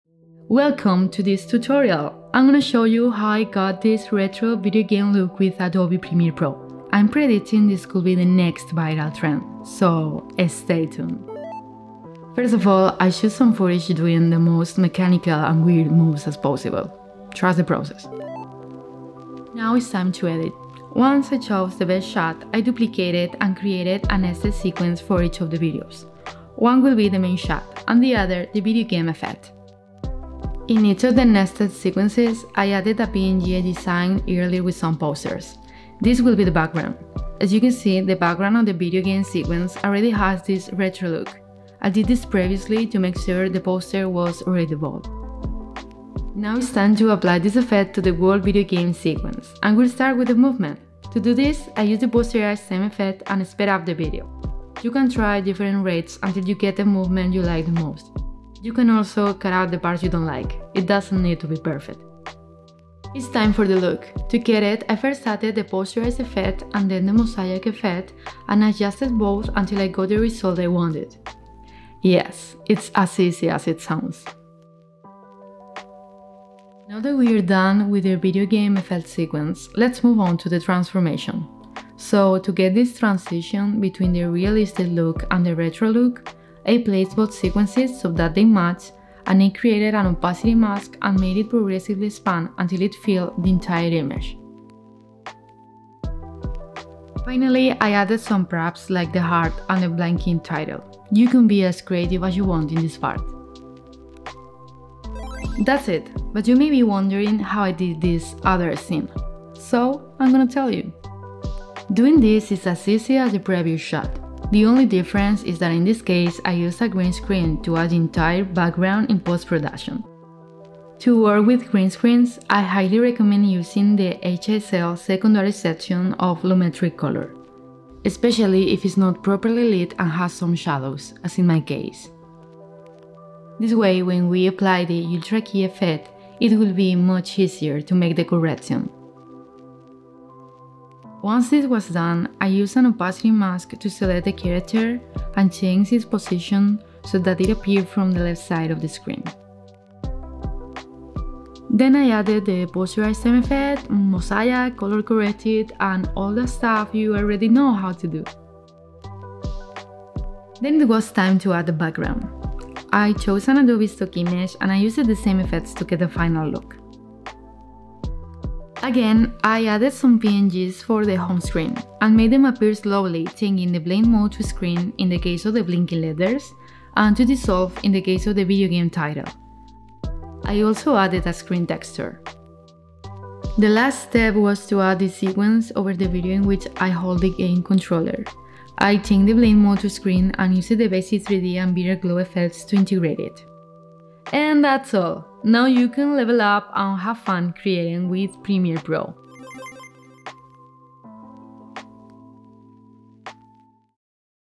Retro Video Game Effect BTS | Premiere Pro x Beatrice Harrods | Year In Premiere 2023 | Adobe Video
Learn how to recreate Beatrice Harrods' Retro Video Game Effect in this PremierePro BTS.